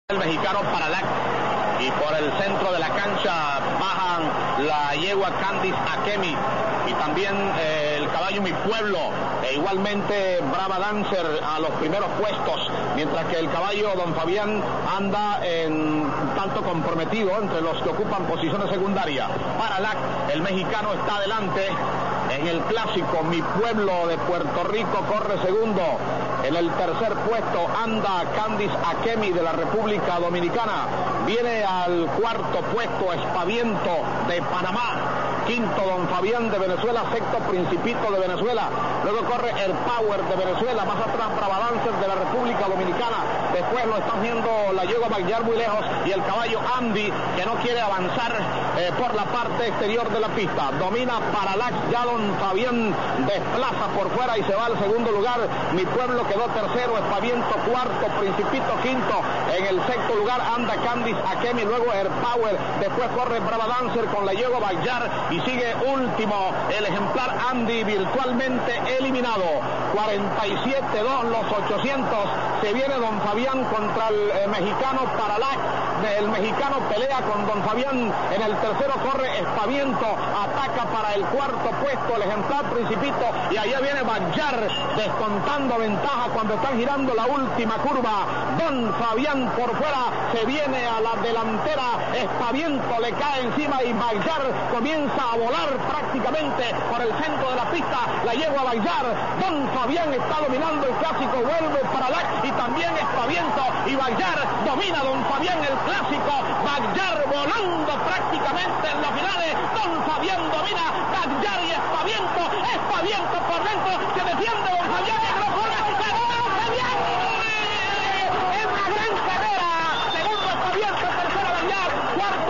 Clasico del Caribe 1990
Cortesía de VTV y Monitor Hípico. Victoria de Don Fabián representando a Venezuela